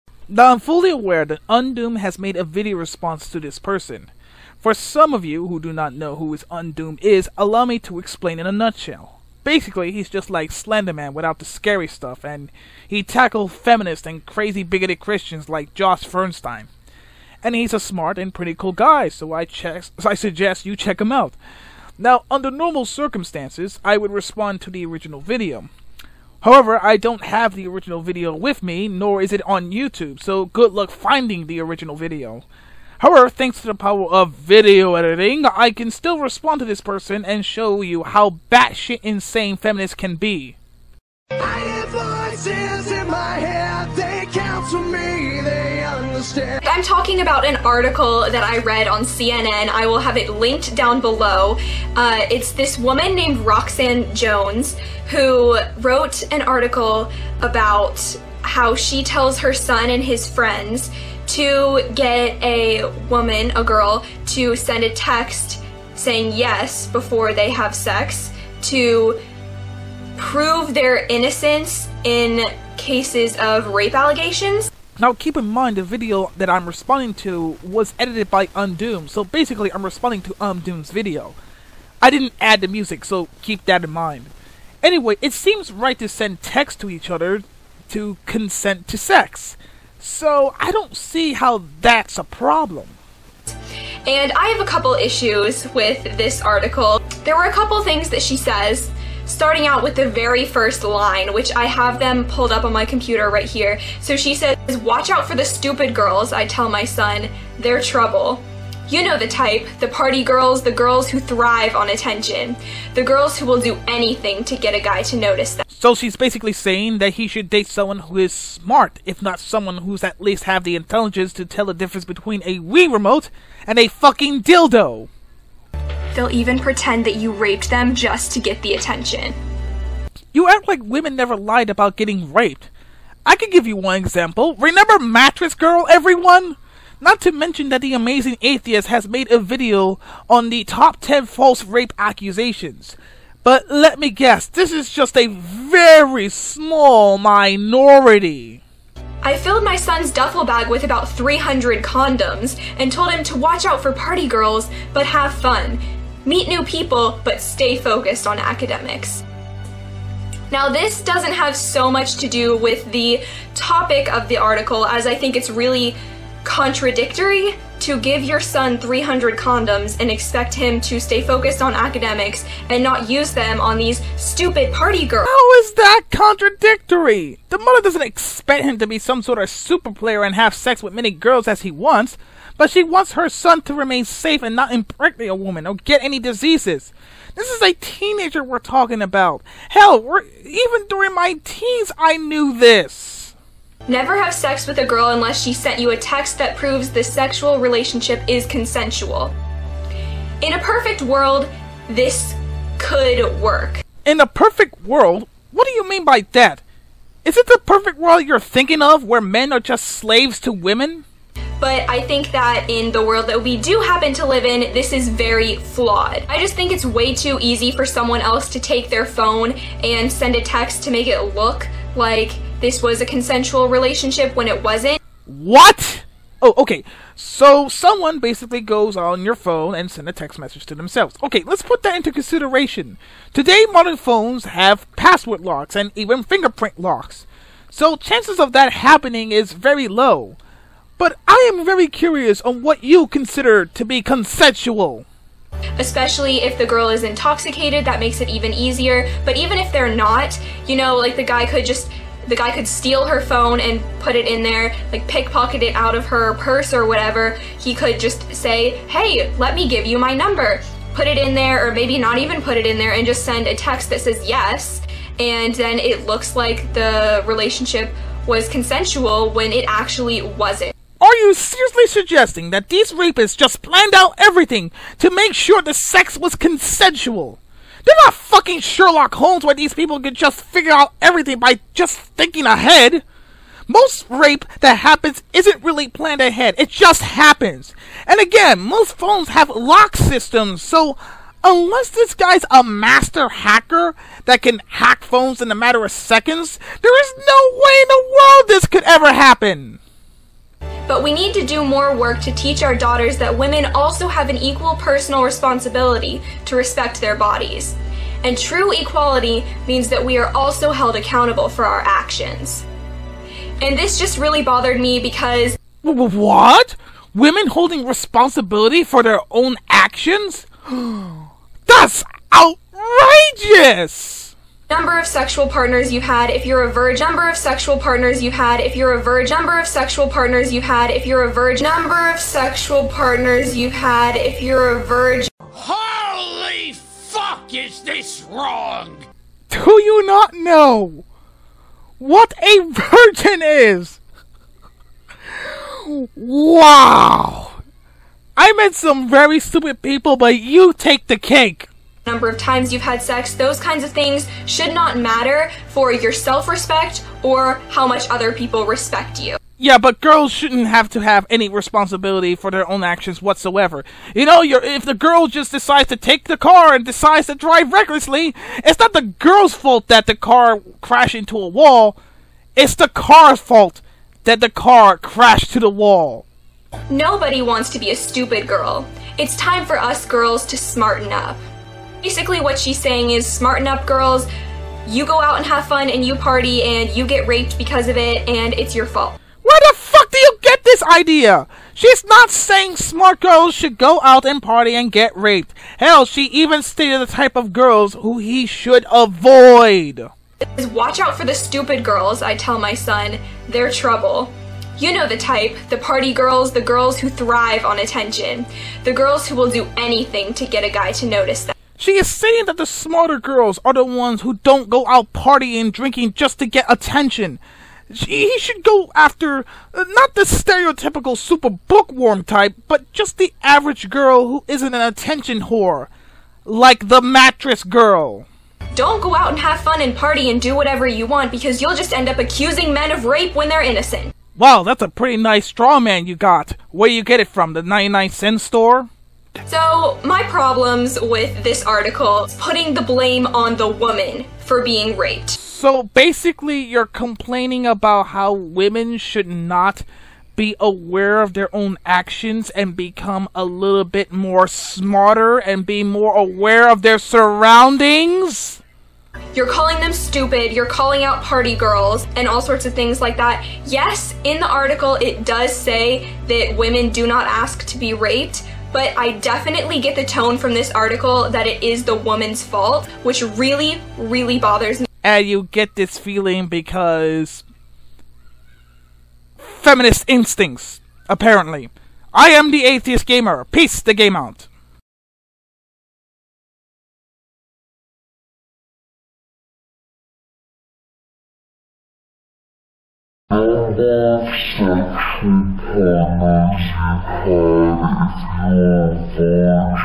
The text is a lie!
Now i do know that undoomed has made a video on her and the original video has been removed however...this person pissed me off to the point where i can't help but just use the clips from undoomed to make my video responce to her. And trust me when i say this you will end up banging your head through tables to hear the amount of stupid this girl has to say.